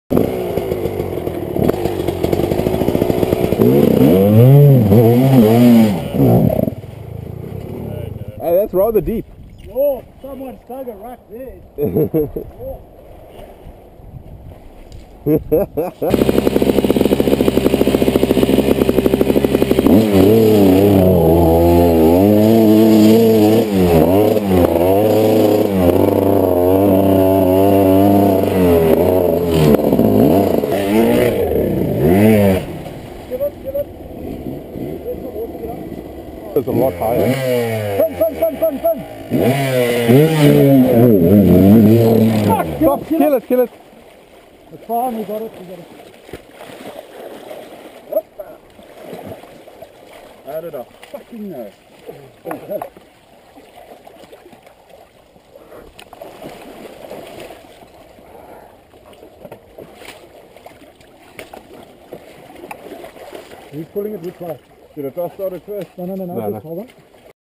Kwelera River Crossing at Rock Rabbit
aka The Drowning of General G
A drop-in to a river section at Rock Rabbit